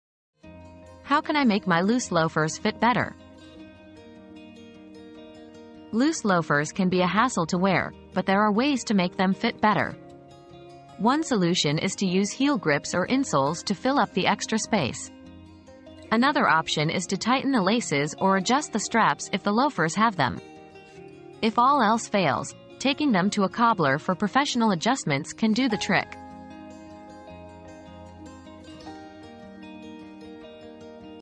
How can I make my loose loafers fit better?
Say goodbye to the frustration of slipping and sliding in your loose loafers! In this video, we'll show you simple and easy ways to make your loafers fit like a glove. From heel grips to professional adjustments, we've got you covered. Don't let ill-fitting shoes ruin your day - watch now and step out in style!